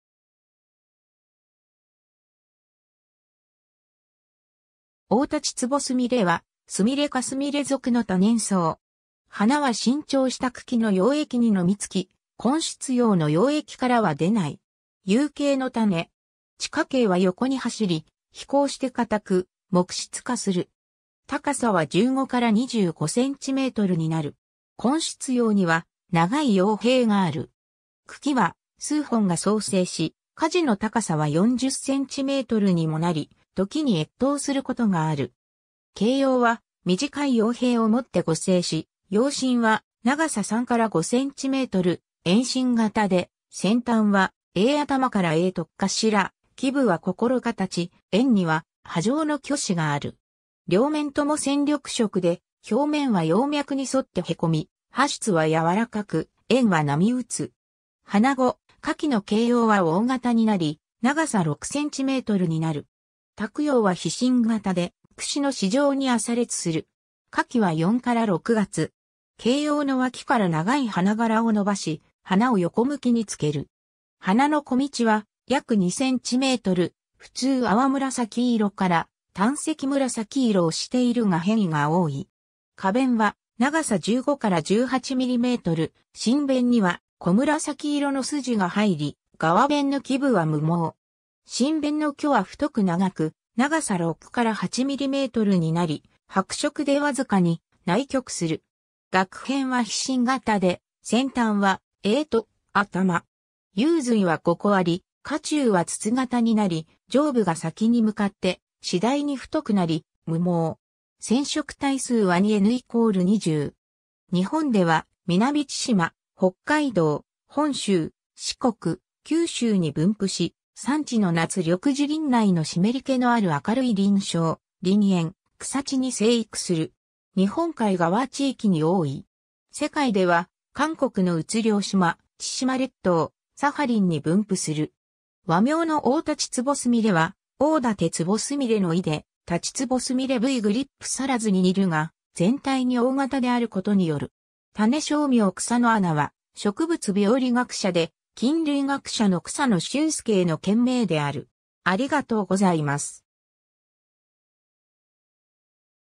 オオタチツボスミレ
オオタチツボスミレ（大立坪菫、学名：Viola kusanoana）はスミレ科スミレ属の多年草。
花は伸長した茎の葉腋にのみつき、根出葉の葉腋からは出ない。
 有茎の種。
地下茎は横に走り、肥厚して硬く、木質化する。
高さは15-25cmになる。
根出葉には長い葉柄がある。
茎は数本が叢生し、果時の高さは40cmにもなり、ときに越冬することがある。
茎葉は短い葉柄をもって互生し、葉身は長さ3-5cm、円心形で、先端は鋭頭から鋭突頭、基部は心形、縁には波状の鋸歯がある。
両面とも鮮緑色で、表面は葉脈に沿ってへこみ、葉質はやわらかく、縁は波打つ。
花後、夏期の茎葉は大型になり、長さ6cmになる。
托葉は披針形で、くしの歯状に浅裂する。
茎葉の腋から長い花柄を伸ばし、花を横向きにつける。
花の径は約2cm、ふつう淡紫色から淡赤紫色をしているが変異が多い。
花弁は長さ15-18mm、唇弁には濃紫色のすじが入り、側弁の基部は無毛。
唇弁の距は太く長く、長さ6-8mmになり、白色でわずかに内曲する。
萼片は披針形で、先端は鋭突頭。
雄蕊は5個あり、花柱は筒形になり、上部が先に向かって次第に太くなり、無毛。
染色体数は2n=20。
 日本では、南千島、北海道、本州、四国、九州に分布し、山地の夏緑樹林内の湿り気のある明るい林床、林縁、草地に生育する。
日本海側地域に多い。
世界では、韓国の鬱陵島、千島列島、サハリンに分布する。
 和名のオオタチツボスミレは「大立坪菫」の意で、タチツボスミレ（立坪菫） V. grypoceras に似るが、全体に大型であることによる。
 種小名（種形容語）kusanoana は、植物病理学者で菌類学者の草野俊助 (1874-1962) への献名である。